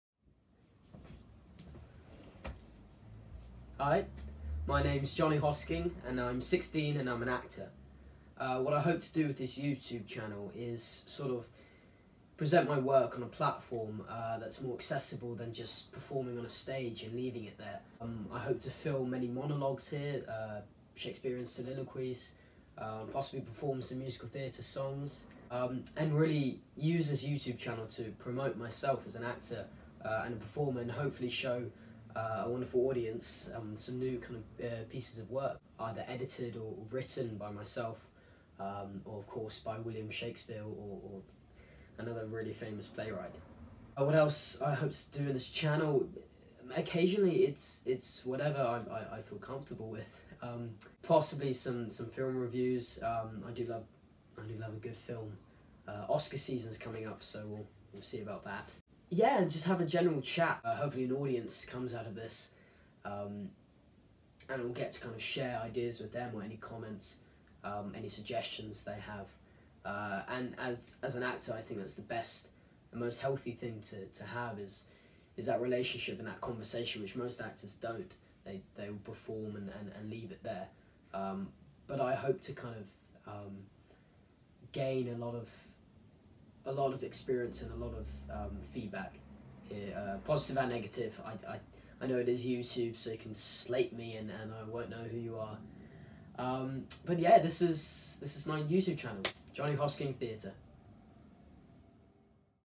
'JONNY HOSKING THEATRE' Introduction
Hi, I'm Jonny Hosking and I'm an actor.

This is just a brief introduction/overview of what I intend for this channel to be. Here's to an exciting future!